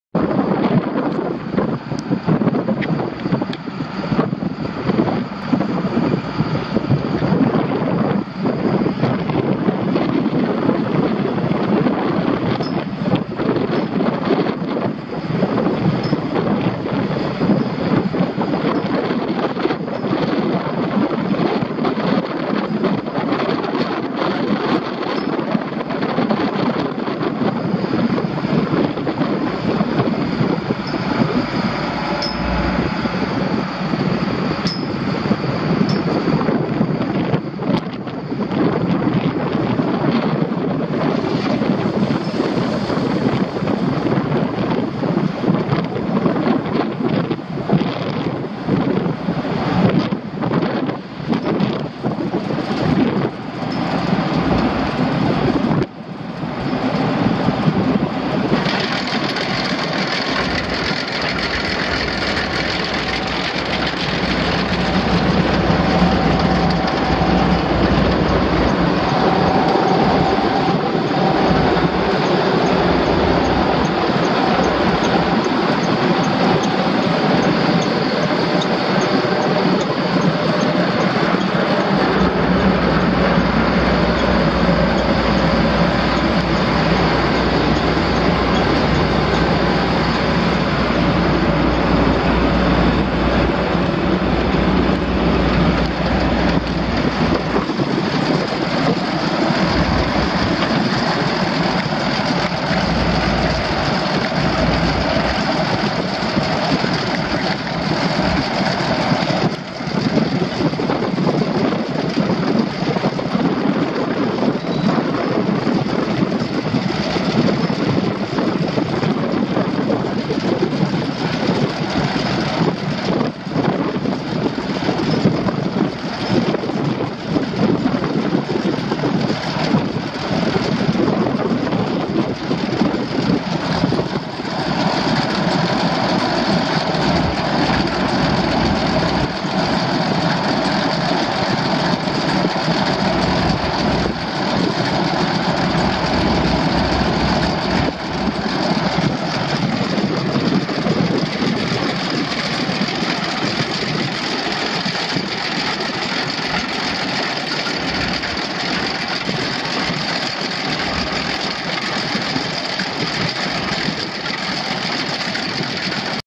2001 Caterpillar Lexion 470 combine for sale at auction | bidding closes October 9, 2019
2001 Caterpillar Lexion 470 combine
2,327 engine hours on meter
Caterpillar 3126 7.2L six cylinder diesel engine
290 HP
Hydrostatic transmission
3F-3R gears
Feederhouse
Variable speed
Reverser
AC and heat
Small wire concaves
Two speed rotor
Chaff spreader
Straw chopper
Power folding bin
Yield monitor
18.4R38 dual front tires
500/60-26.5 rear tires
Serial 9TW00444
It's located in Stratford, TX, and we're selling it to the highest bidder regardless of price.